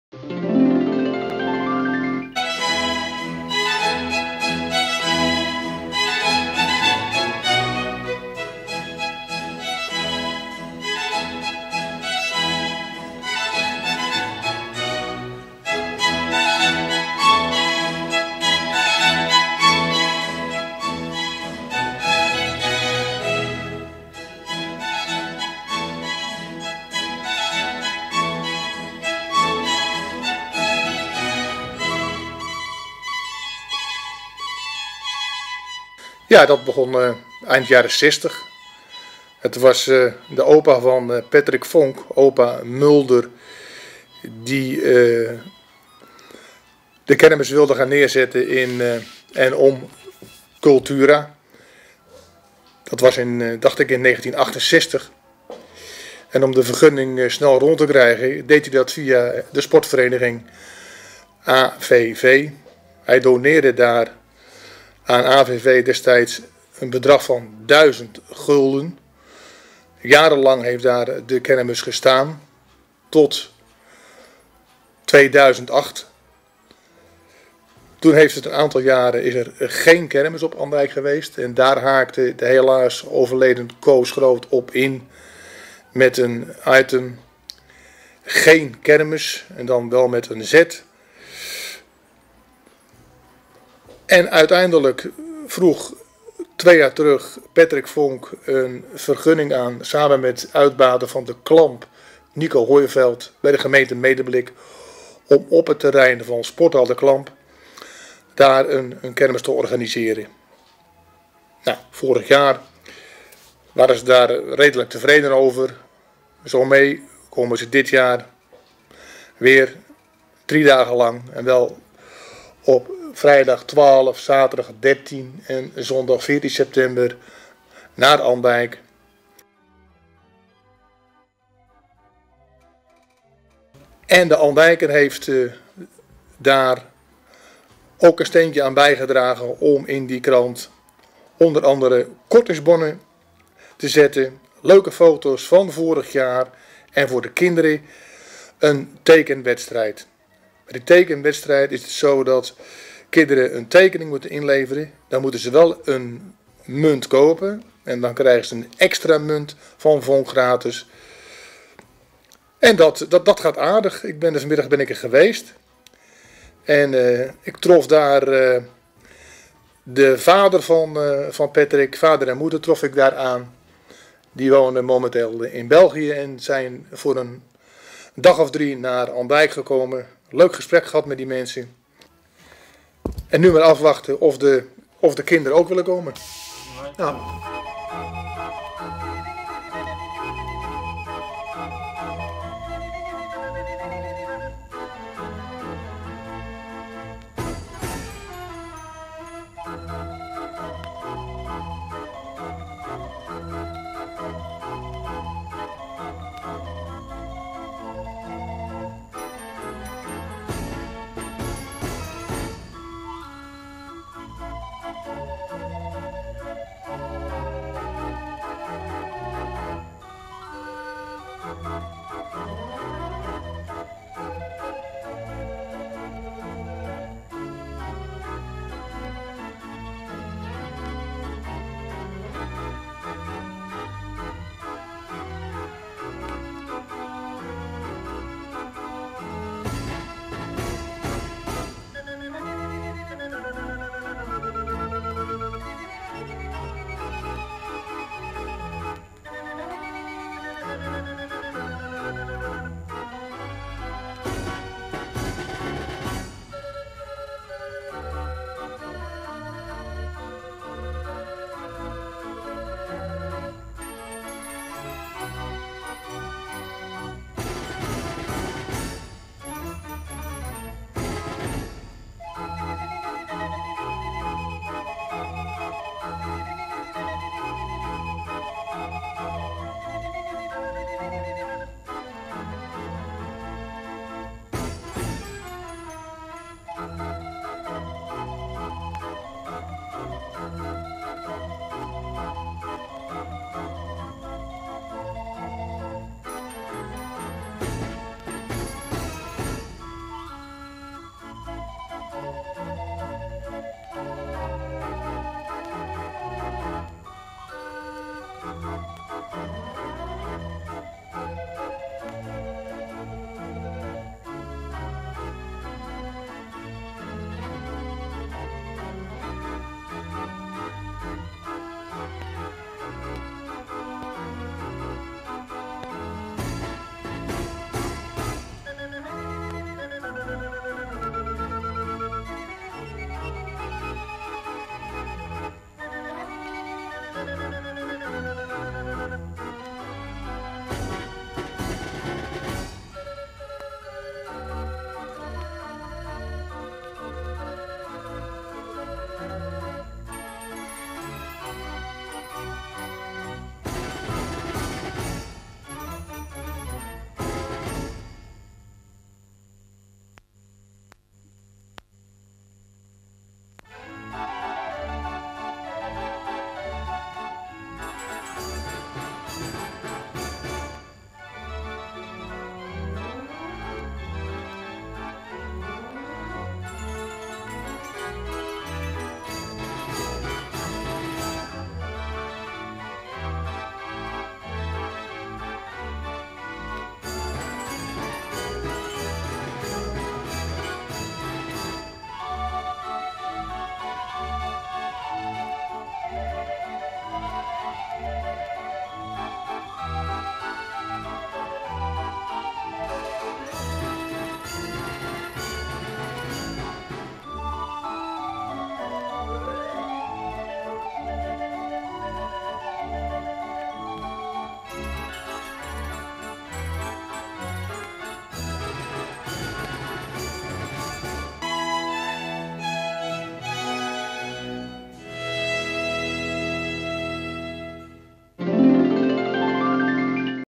ANDIJK KERMIS
voor de tweede keer een kermis bij De Klamp  in Andijk